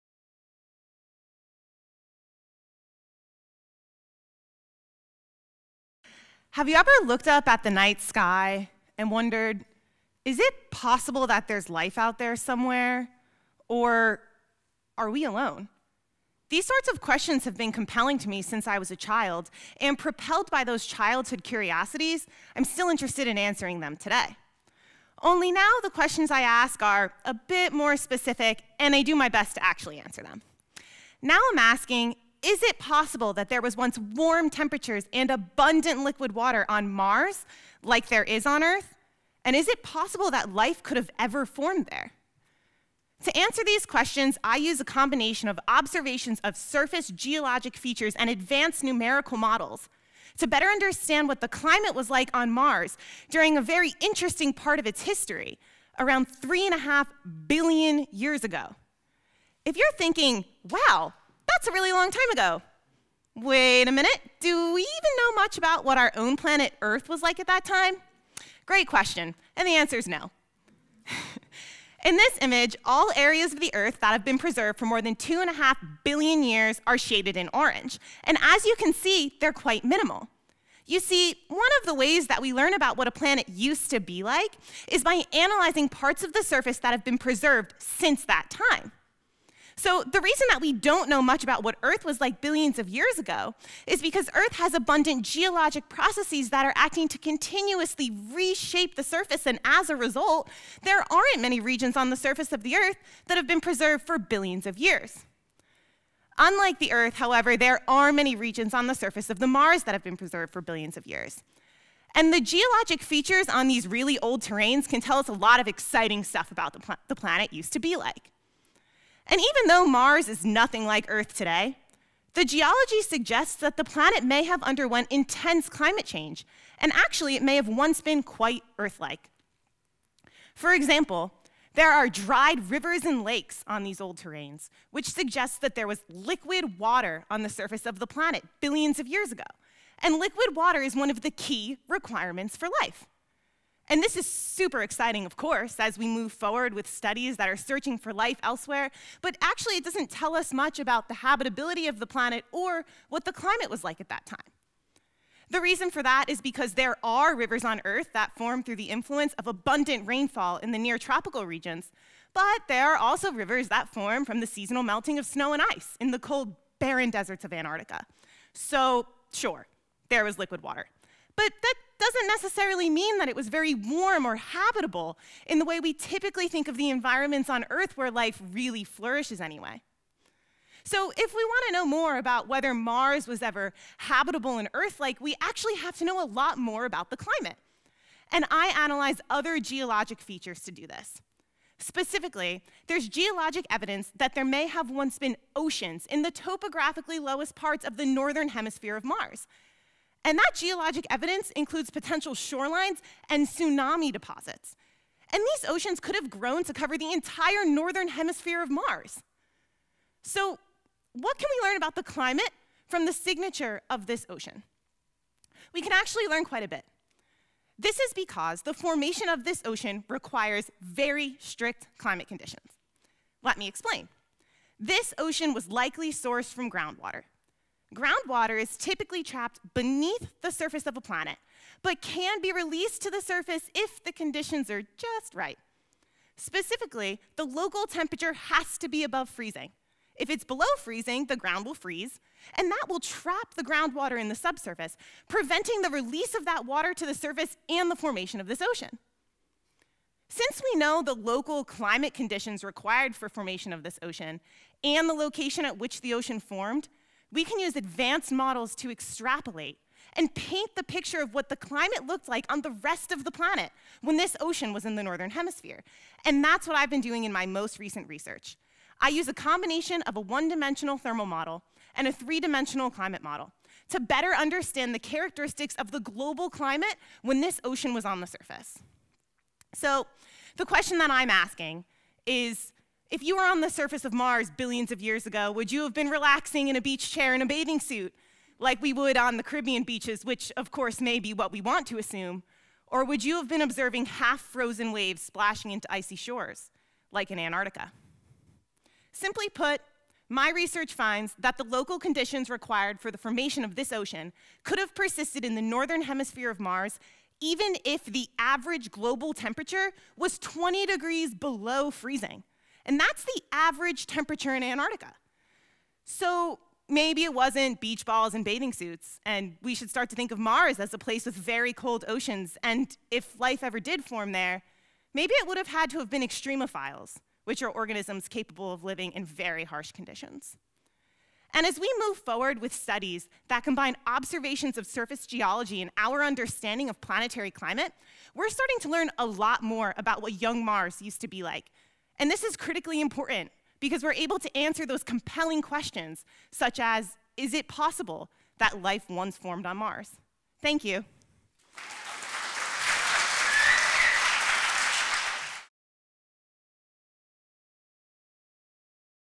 Ashley Palumbo: Oceans on Early Mars
Ashley Palumbo, PhD Candidate in Earth, Environmental, and Planetary Sciences and a NASA Earth and Space Science Fellow, talks about her research on the possibility of oceans on Mars. She explores through the use of advanced modeling, how we can better understand what the climate was like during this time in the planet’s history.

This talk was part of Research Matters, featuring short talks about research by Brown University Graduate Students on April 9, 2019.

Brown University